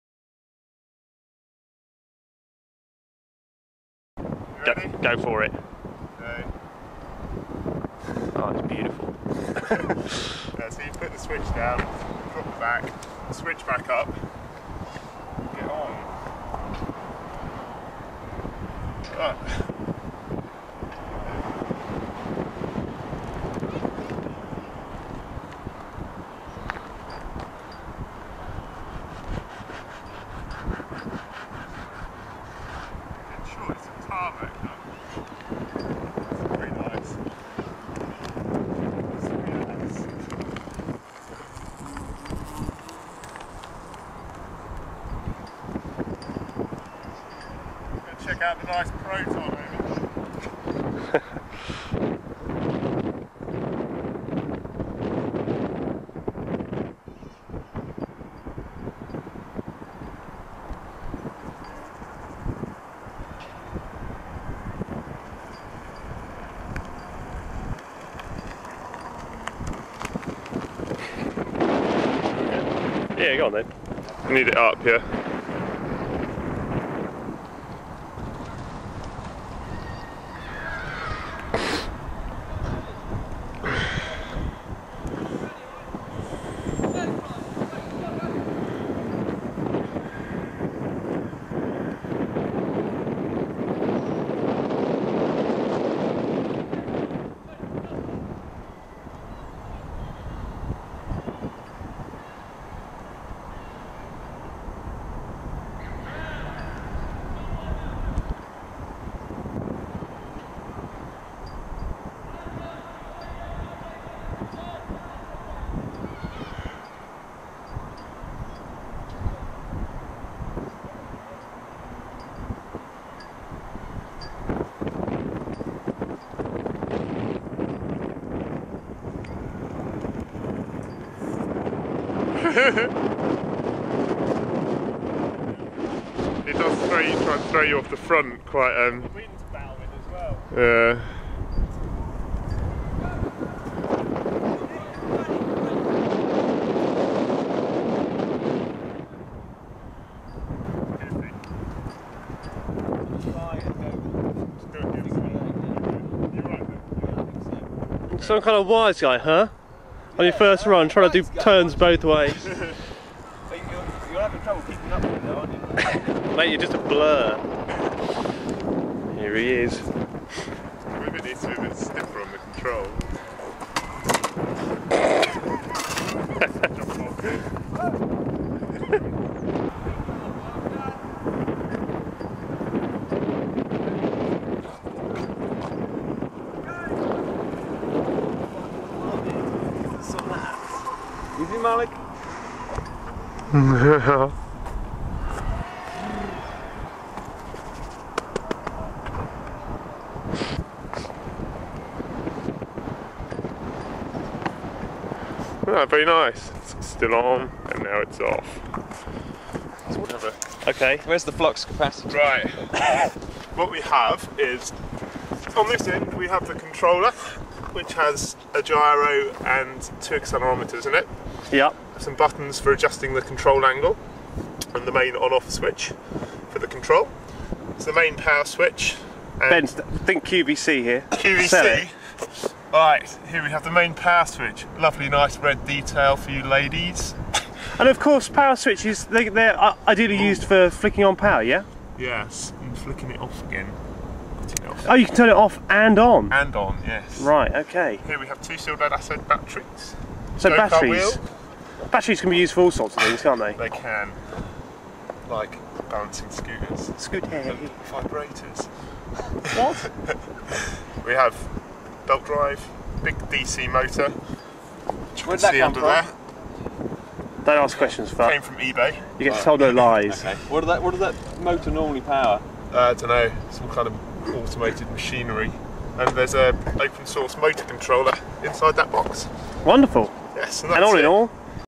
Balancing Scooter / Skateboard
A balancing scooter using a go-kart wheel, a 400 Watt 24V DC Motor, an OSMC and a PIC microcontroller.  A simple PID control at 100 Hz balances the assembly.